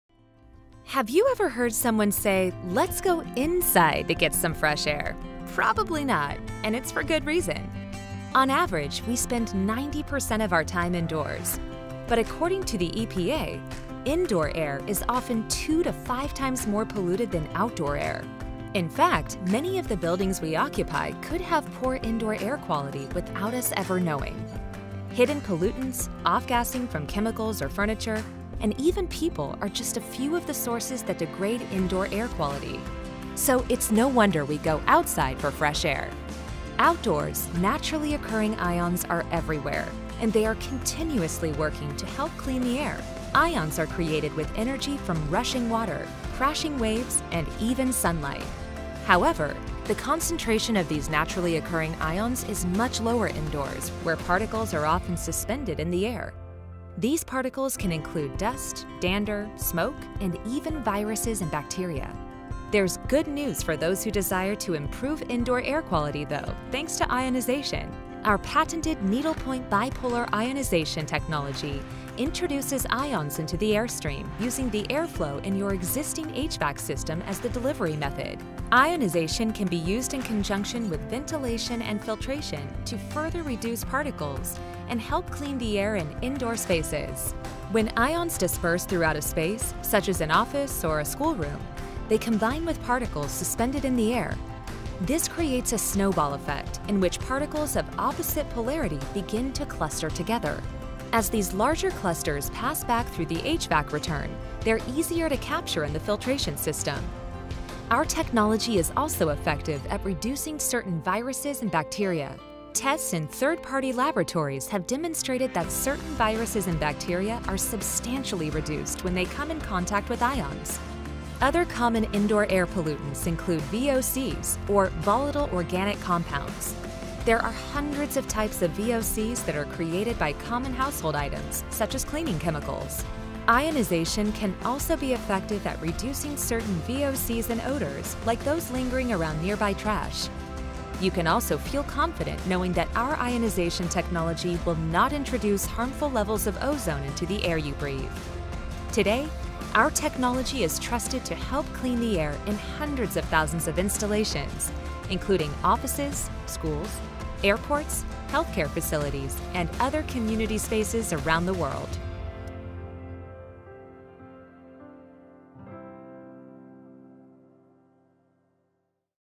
GPS Power of Ions
GPS’ patented NPBI® technology helps clean the air by introducing ions into the space via the airflow in your ventilation system. Our products improve the air by reducing airborne particles, including certain odors, viruses and bacteria without introducing ozone.